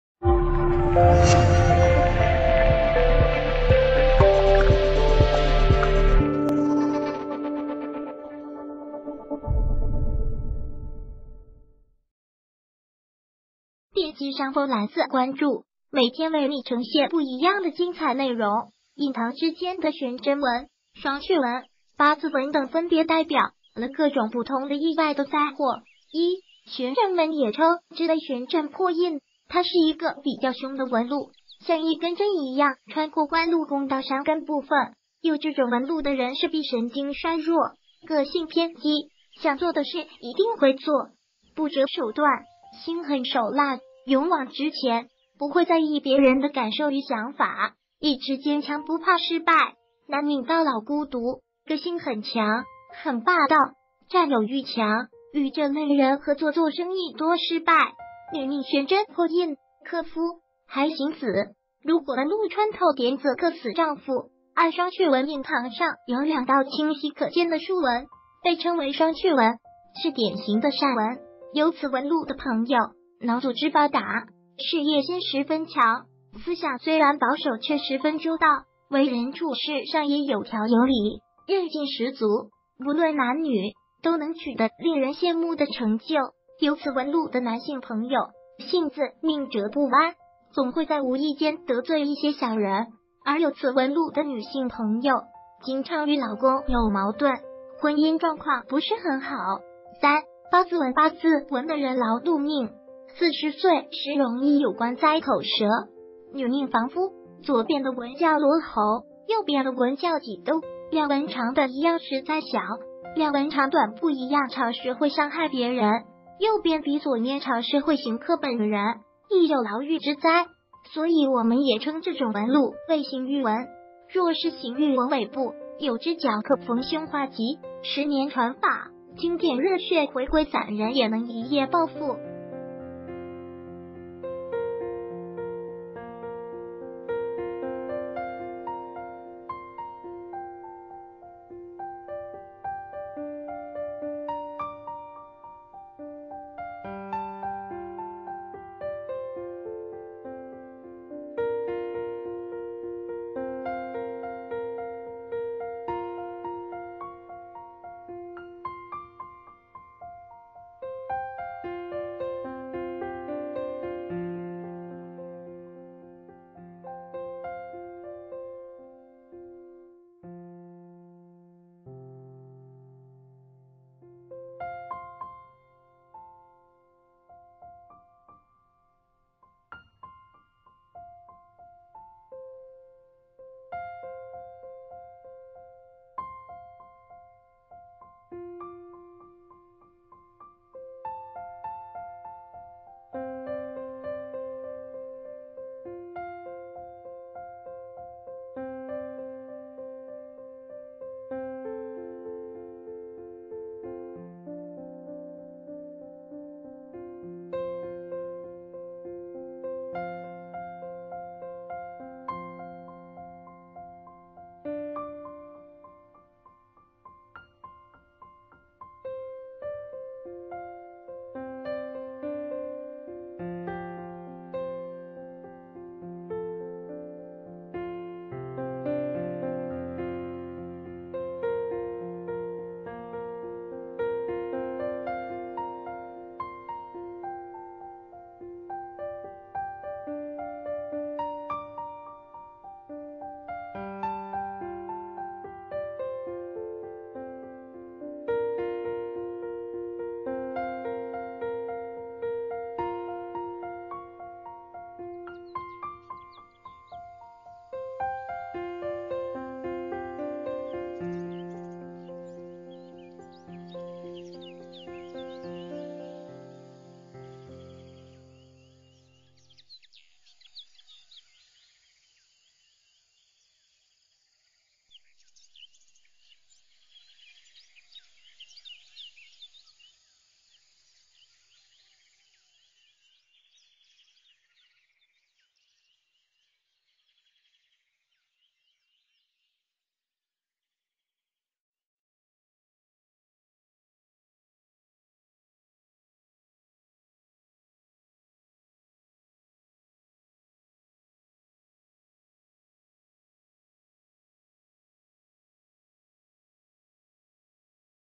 脸上有一纹，可看生死
谢谢！点击上方“蓝字”关注,每天为你呈现不一样的精彩内容！印堂之间的悬针纹、双雀纹、八字纹等分别代表了各种不同的意外的灾祸。1、悬针纹也称之为悬针破印，他是一个比较凶的纹路，像一根针一样穿过官禄宫到山根部份，有这种纹路的人势必神经衰弱，个性偏激，想做的事一定会做，不折手段，心狠手辣，勇往直前，不会在意别人的感受与想法，毅志坚强不怕失败。男命到老孤独，个性很强，很霸道，占有欲强，与这类人合作做生意多失败。女命悬针破印，克夫，还刑子，如果纹路穿透典子克死丈夫。2、双雀纹印堂上有两道清晰可见的竖纹，被称为双雀纹，是典型的善纹。有此纹路的朋友，脑组织发达，事业心十分强，思想虽然保守却十分周到，为人处世上也有条有理，韧劲十足，不论男女，都能取得令人羡慕的成就。有此纹路的男性朋友，性子宁折不弯，总会在无意间得罪一些小人。而有此纹路的女性朋友，经常与老公有矛盾，婚姻状况不是很好。3、八字纹八字纹的人劳碌命，四十岁时容易有官灾口舌，女命妨夫。左边的纹叫罗喉，右边的纹叫几都。两纹长短一样时灾小，两纹长短不一样长时会伤害别人，右边比左面长时会刑克本人，易有牢狱之灾。所以我们也称这种纹路为刑狱纹，若是刑狱纹尾部有支脚可逢凶化吉。十年传霸经典热血回归散人也能一夜暴富